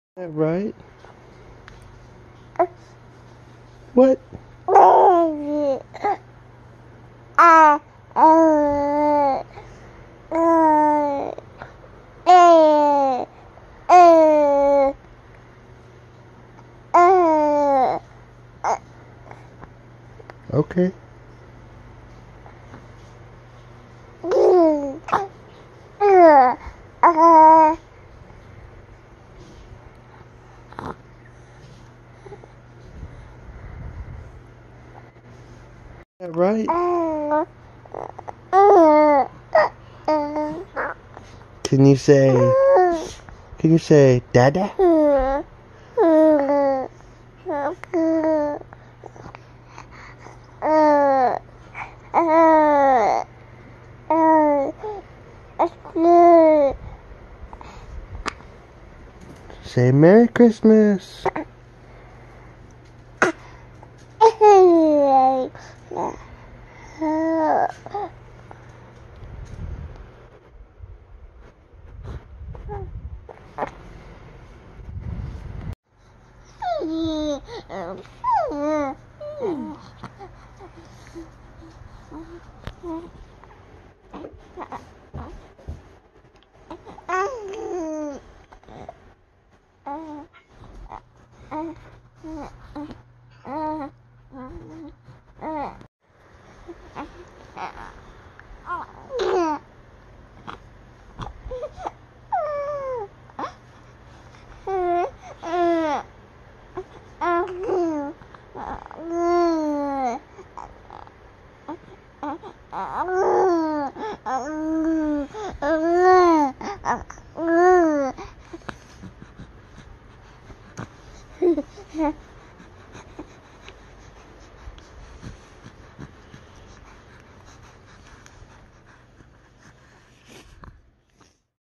Beau Talking in Bed w Dada and Micah | December 2021
Beau Wesley Prichard
Beau Prichard
Jessica Prichard
Ray Prichard
Micah Prichard
December 05, 2021
2 Months Old
Talking Kooing Baby Talk